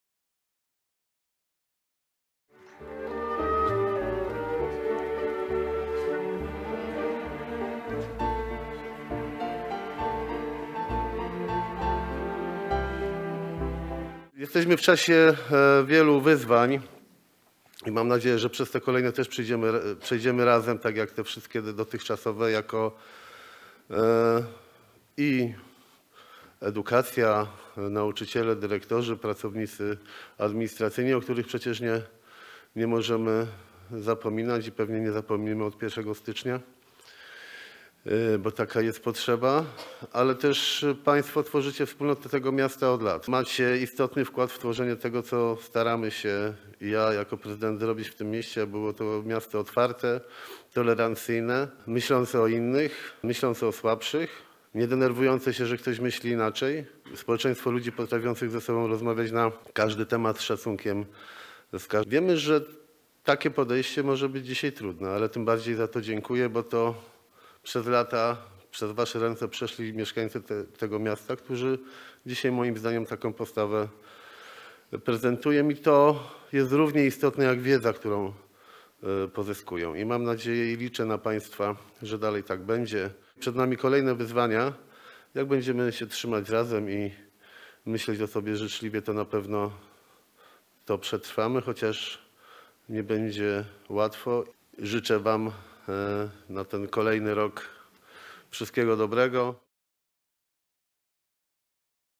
Edukacja to inwestycja
W Teatrze Polskim w Bielsku-Białej 17 października odbyła się uroczysta gala z okazji Dnia Edukacji Narodowej. Ponad 100 nauczycieli różnego szczebla edukacji odebrało z rąk prezydenta Bielska-Białej Jarosława Klimaszewskiego nagrody.